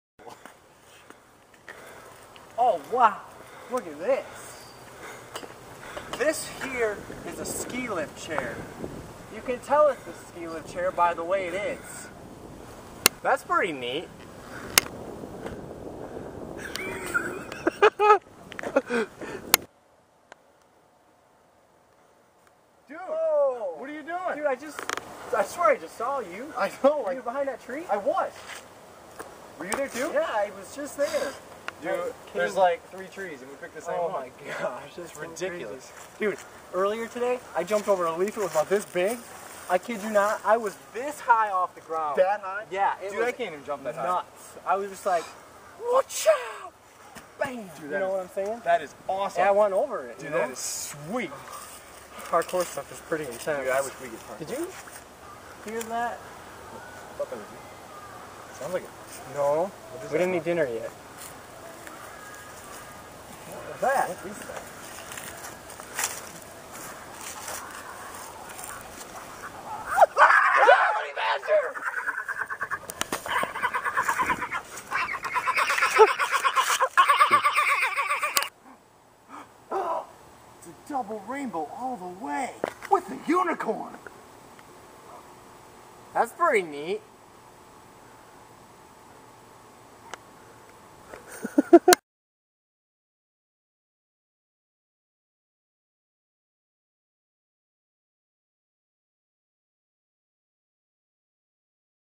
Parody of A Parody of A Parody.wmv
Parody of Double Rainbow, Honey Badger, and Charlie the unicorn